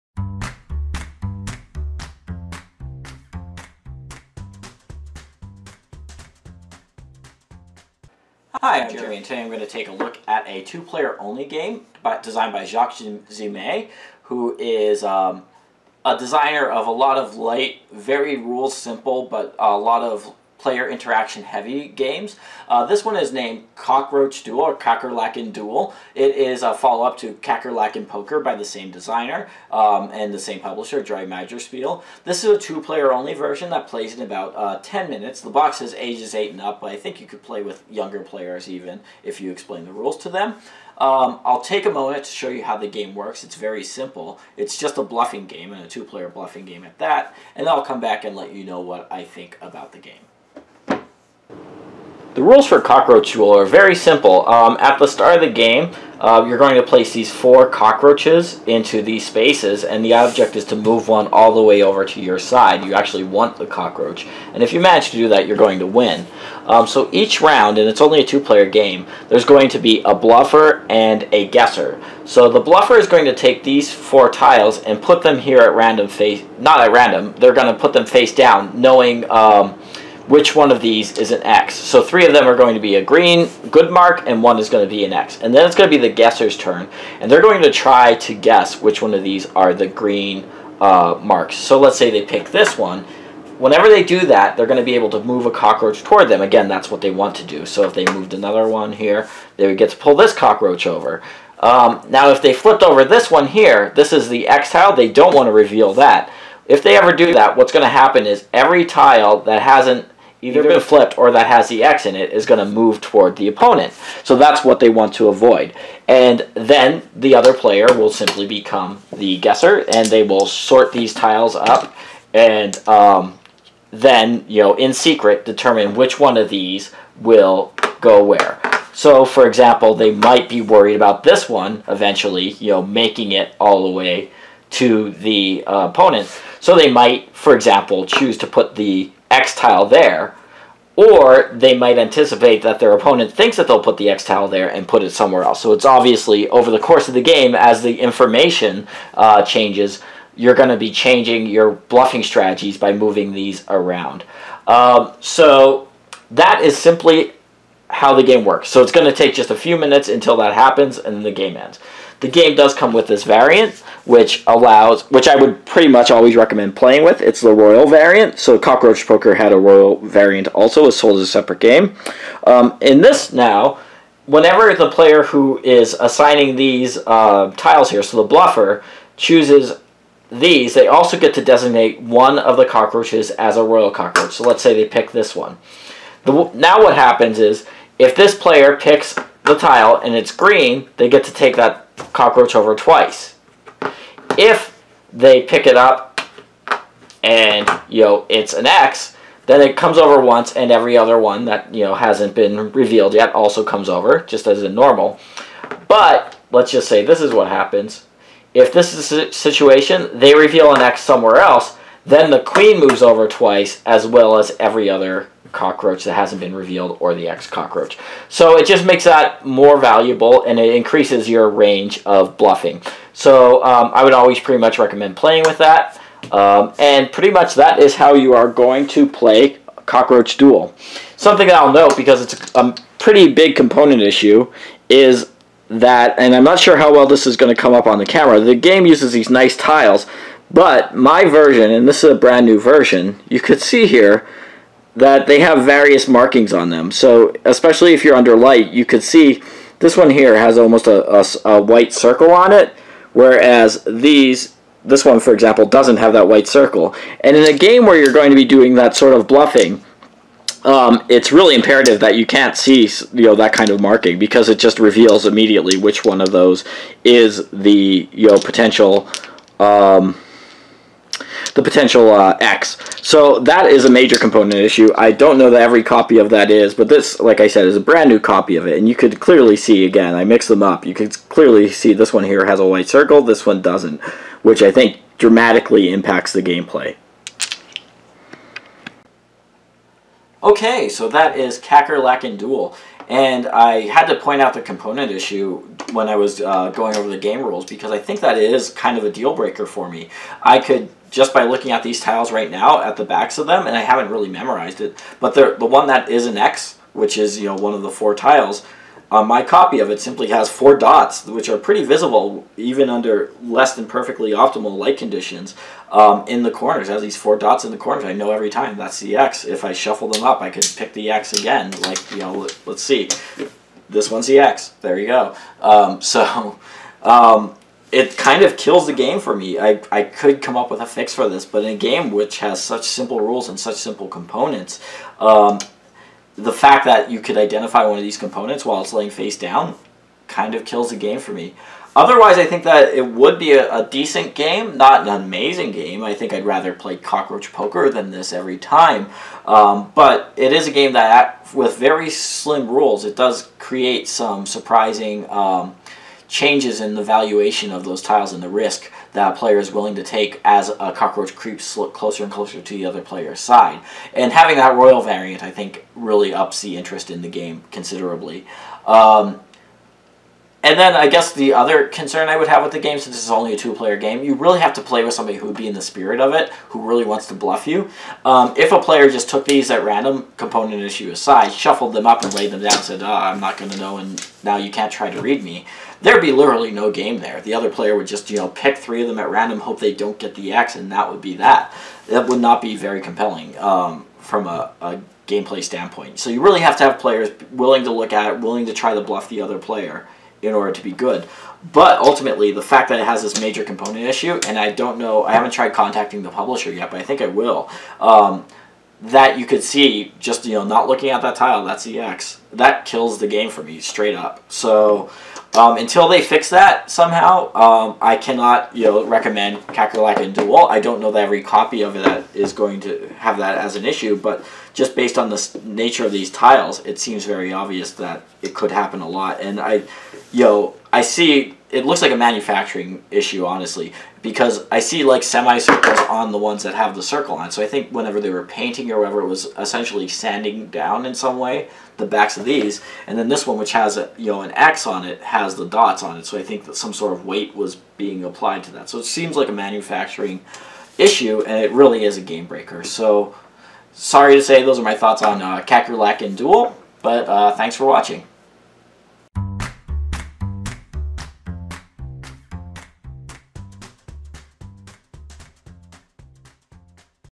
Jeremy Reviews It... - Kakerlaken Duell Board (Cockroach Duel) Game Review
Board game review and rules demonstration for Kakerlaken Duell Board (Cockroach Duel) (2017), a two-player bluffing board game designed by Jacques Zeimet.